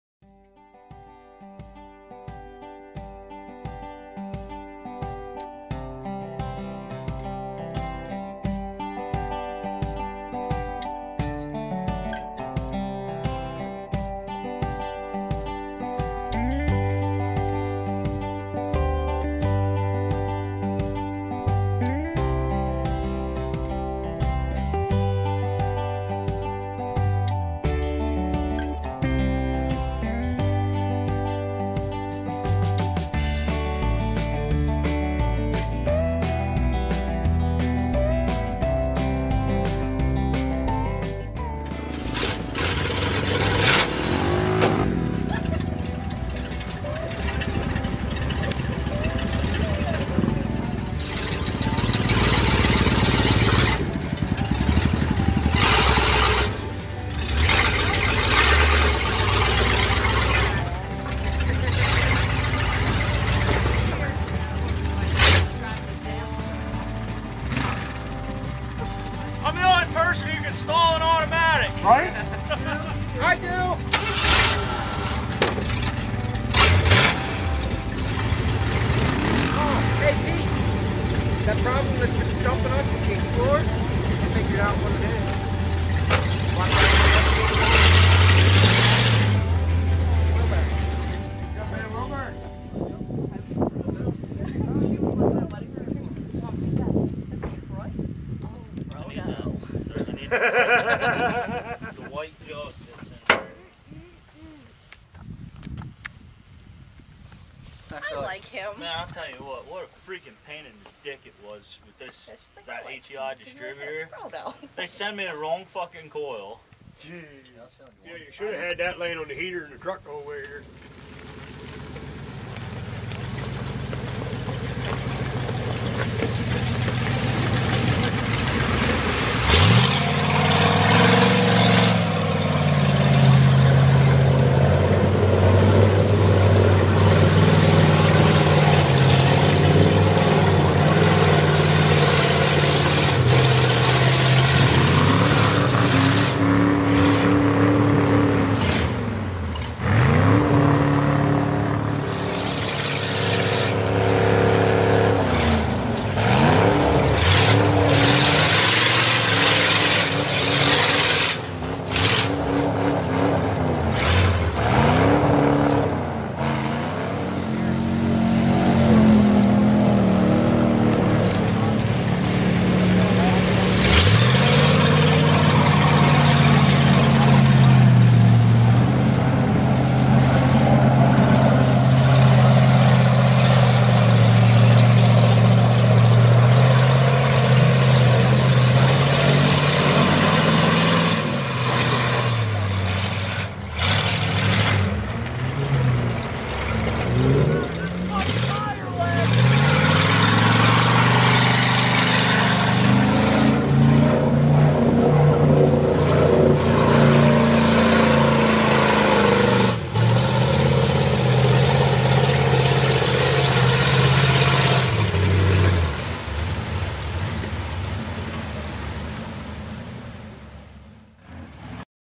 CHARLES FORD SHORT BED ,V8 , GRAND PRIX MUD TIRES
2018 , LAST MUD BOG EVENT OF THE YEAR , HAD CHARLES & HIS V8 FORD SHORT BED AT PLAY IN THE MUD BOG  , AT MODICKERS